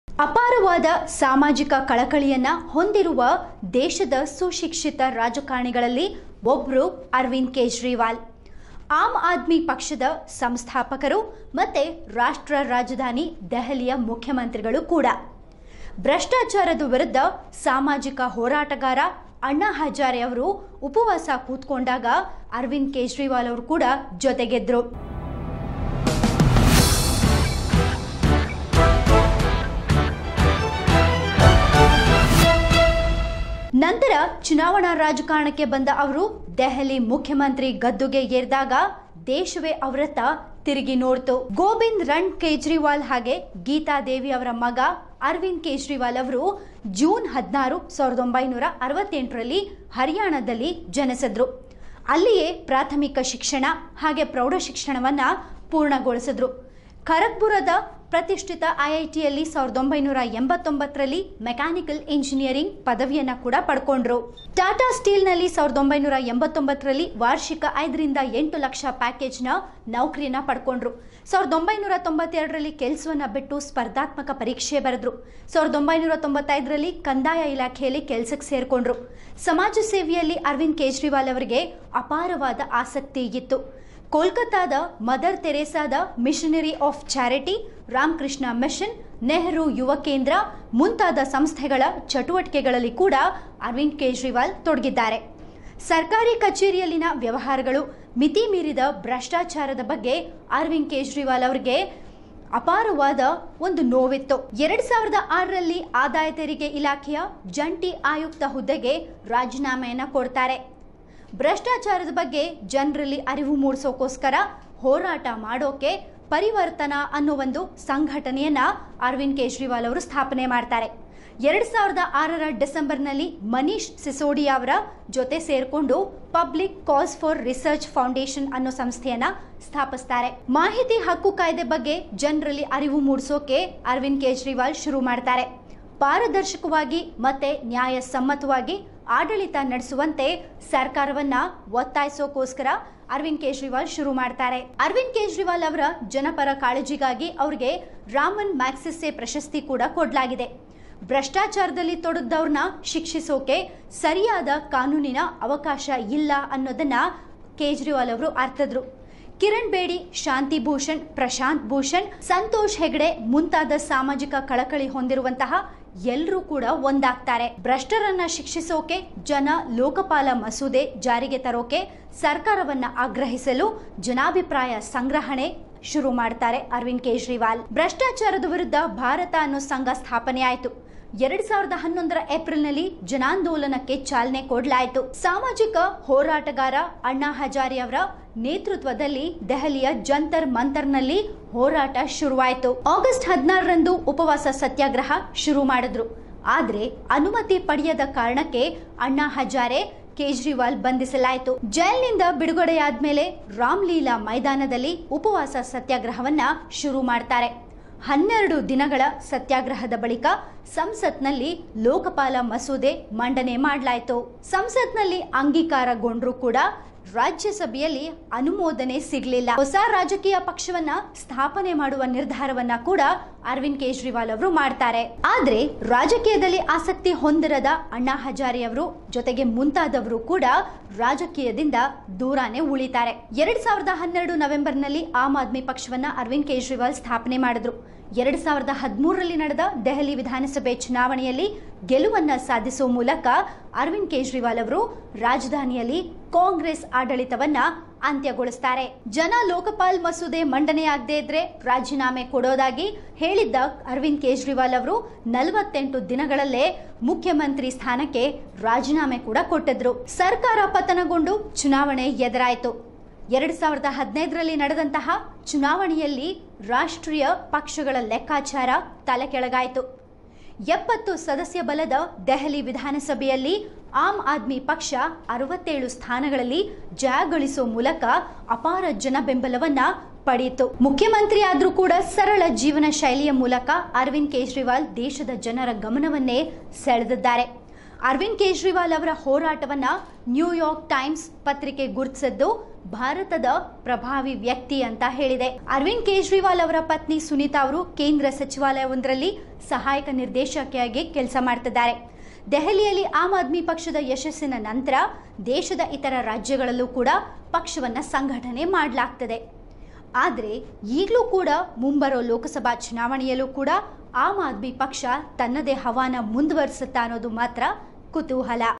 Lok Sabha Elections 2019 : ಅರವಿಂದ್ ಕೇಜ್ರಿವಾಲ್ ವ್ಯಕ್ತಿಚಿತ್ರ | Oneindia Kannada
ಅಪಾರವಾದ ಸಾಮಾಜಿಕ ಕಳಕಳಿ ಹೊಂದಿರುವ ದೇಶದ ಸುಶಿಕ್ಷಿತ ರಾಜಕಾರಣಿಗಳಲ್ಲಿ ಒಬ್ಬರು ಅರವಿಂದ್ ಕೇಜ್ರಿವಾಲ್. ಆಮ್‌ ಆದ್ಮಿ ಪಕ್ಷದ ಸಂಸ್ಥಾಪಕ ಮತ್ತು ರಾಷ್ಟ್ರ ರಾಜಧಾನಿ ದೆಹಲಿಯ ಮುಖ್ಯಮಂತ್ರಿ.

Arvind Kejriwal is one of the country's well-educated politicians with immense social knowledge. Aam Aadmi Party Founder and Chief Minister of Delhi, National Capital. Watch video to know more.